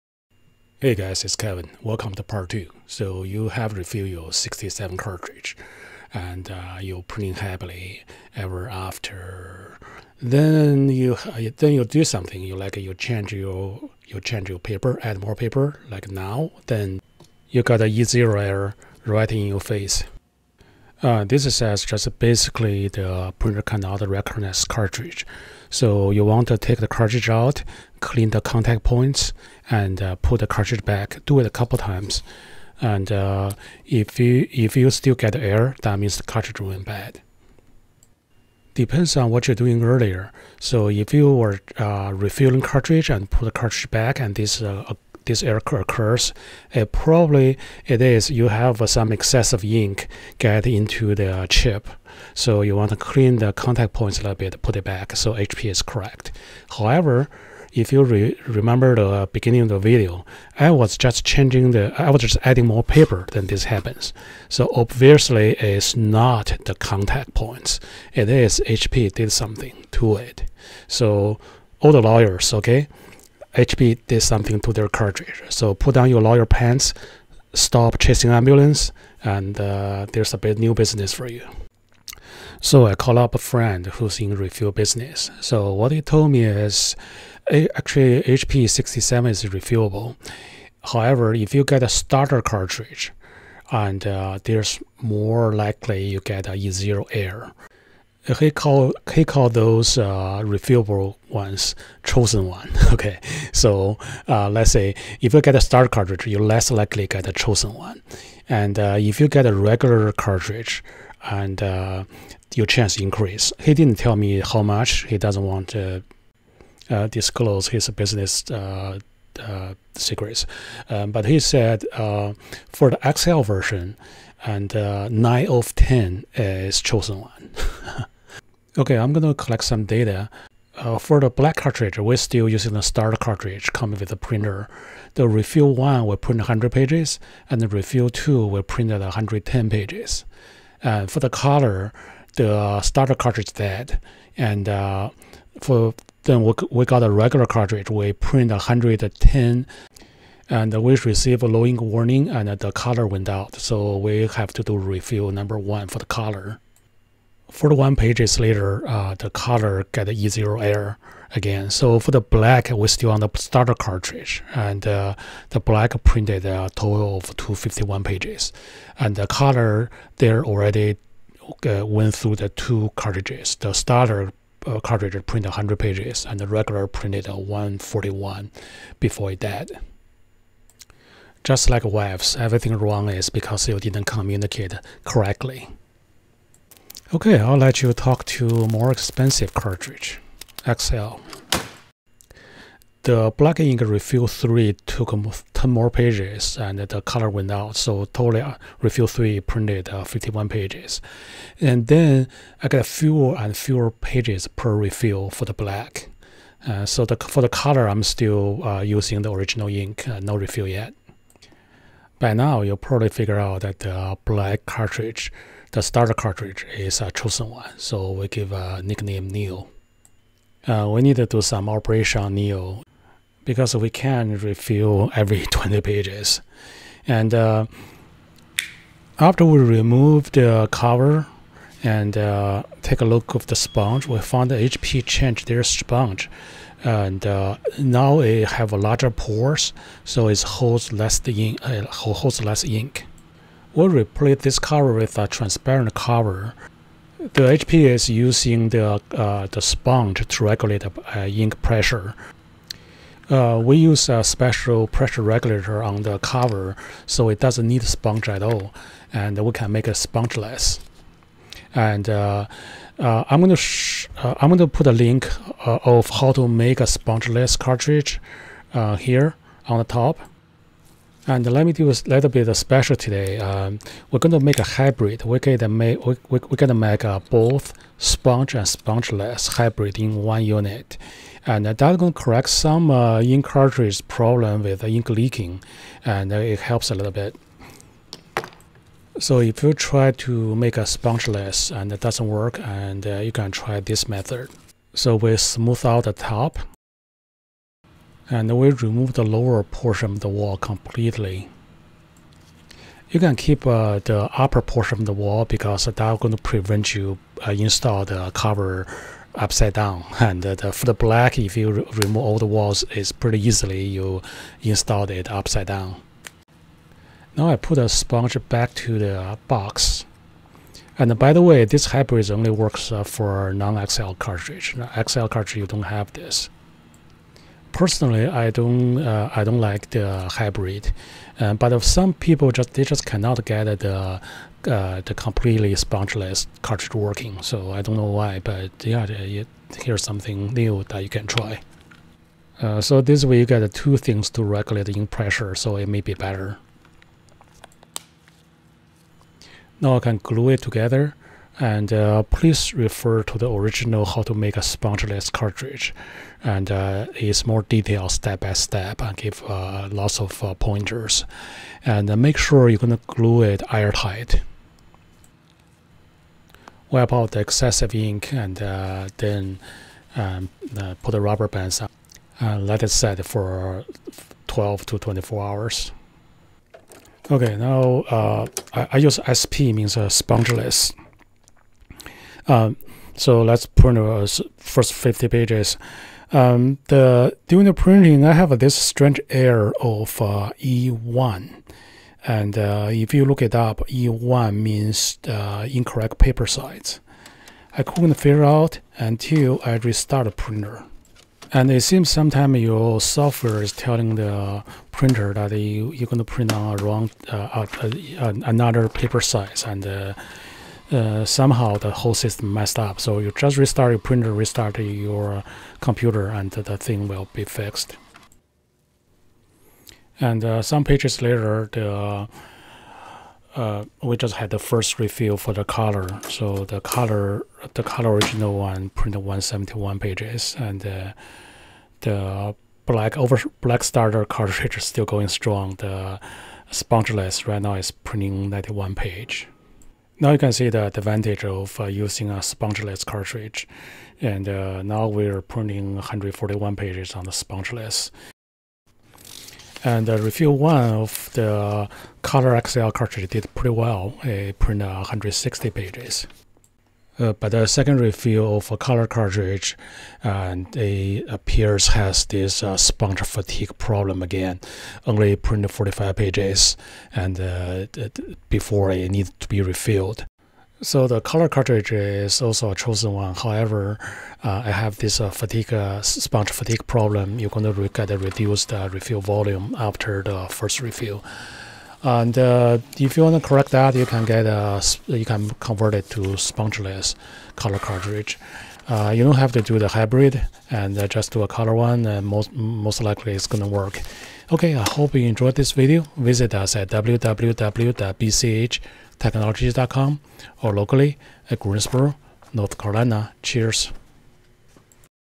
Refill HP 67 67XL Ink Cartridge - Part 2 - E0 E1 Error, Spongeless Refillable & Sponge Fatigue
We deal with refill's common problem of E0 & E1 Error and demonstrated the effect of HP's sponge fatigue in the new 67 cartridge series.  We showed how to make a spongeless cartridge for the black 67 starter cartridge.